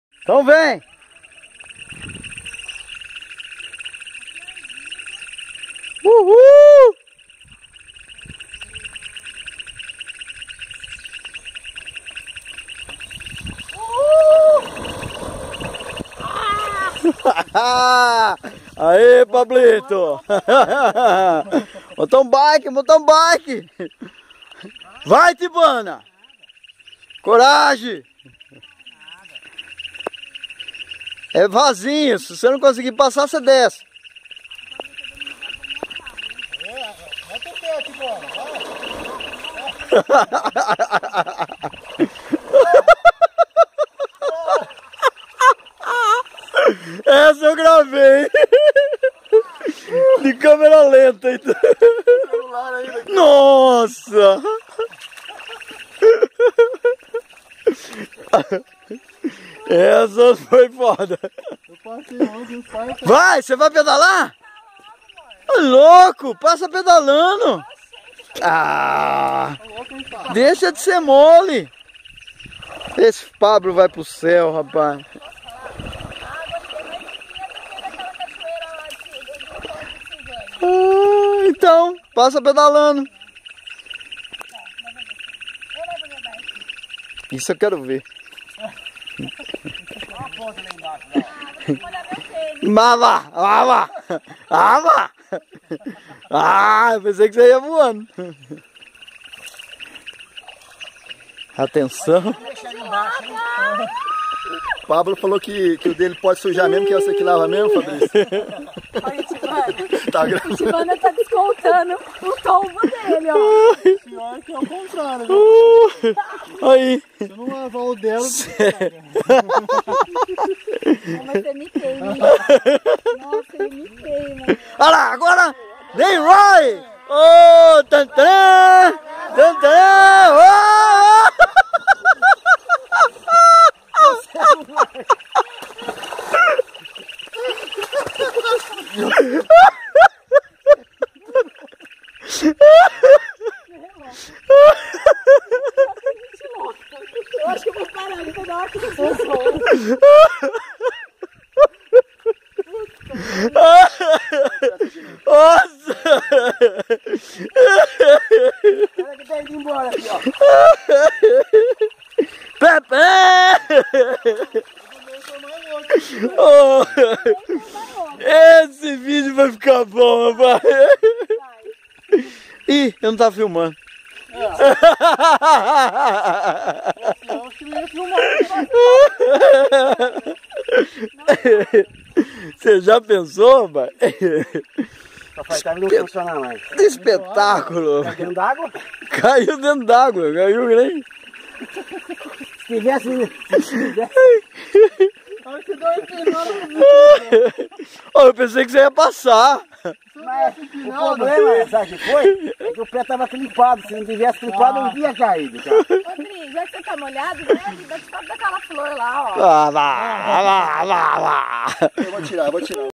PedalAraraquara VD5 - Tombos no Rio
Passeio com a Galera do PedalAraraquara. Passamos por Bueno de Andrada, e seguimos até a Padaria AMA do Assentamentos Monte Alegre. Retornamos pela antiga represa da CODASP, passamos por Silvânia até Bueno e seguimos para Araraquara pelas torres. Na padaria comemos um delicioso bolo de milho com suco de acerola e laranja. Passeio incrível e um ótimo ambiente para encontrar amigos e dar boas risadas em mais uma aventura do PedalAraraquara!